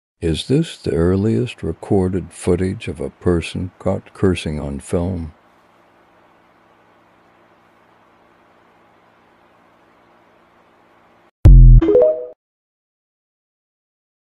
Earliest recording of profanity on film!
Filmed by Mitchell and Kenyon, is this the earliest film made which captures cursing on camera?